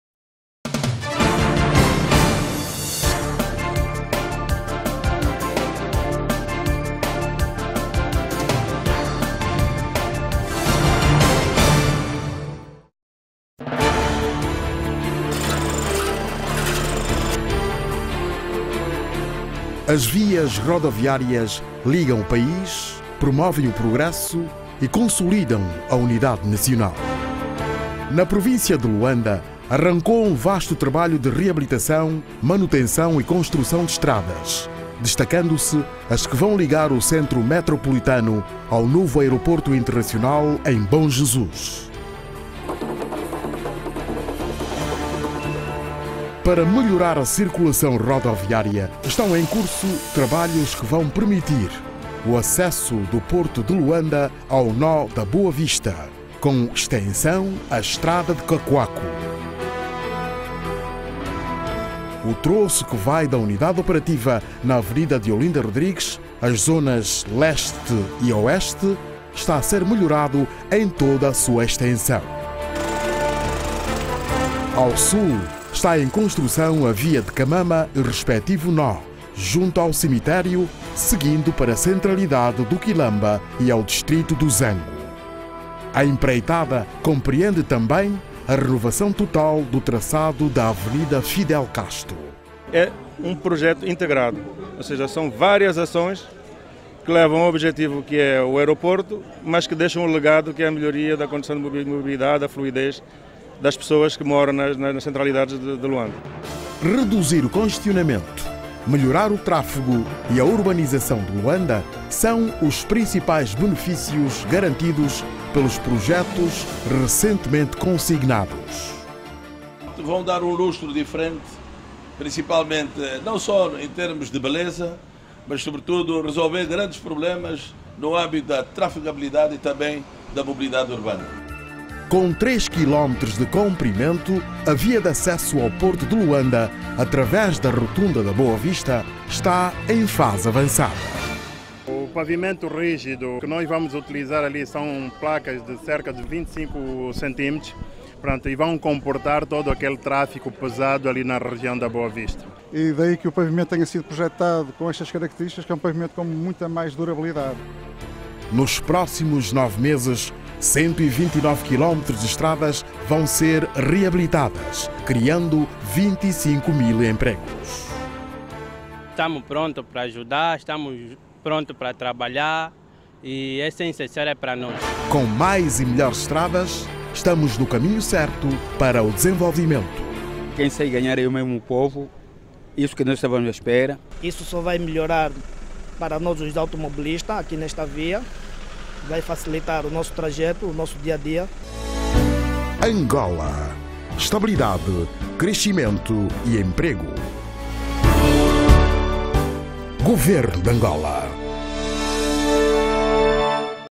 ANGOLA ACONTECE: Vias de Luanda (Road Construction in Luanda) 2017
Angola is on its way to rebuild the country and be a leader in SUb Saharan Africa.
Abundant with natural resources (top oil producer in SSA and more blocks to come online and 4th largest diamond producer worldwide) with many sectors unexplored, never ending fertile land, hydrographic reservoirs a sea full of fish, green field development opportunities and necessities for industries for internal consumption (including real estate).